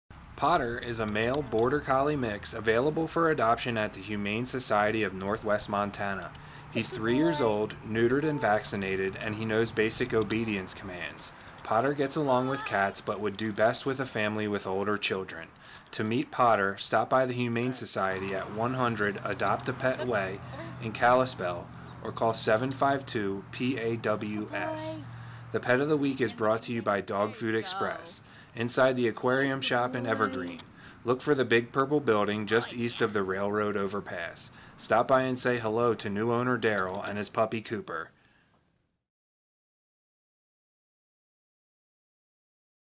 Pet of the Week: Potter (border collie mix) -- April 13, 2018
Potter is a 3-year-old, male border collie mix available for adoption from the Humane Society of Northwest Montana in Kalispell.